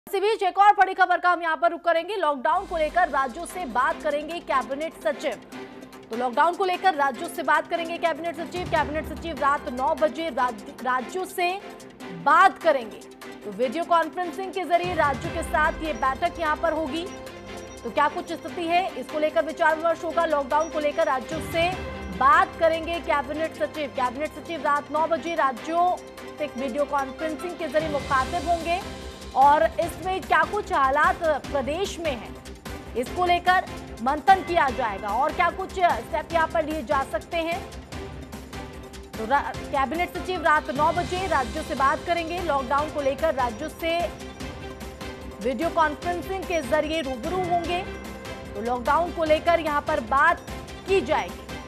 लॉकडाउन 4 को लेकर रात 9 बजे राज्यों से बात करेंगे कैबिनेट सचिव
लॉकडाउन को लेकर राज्यों से बात करेंगे कैबिनेट सचिव
कैबिनेट सचिव रात 9 बजे राज्यों से करेंगे बात
वीडियो कॉन्फ्रेंसिंग के जरिए राज्यों के साथ बैठक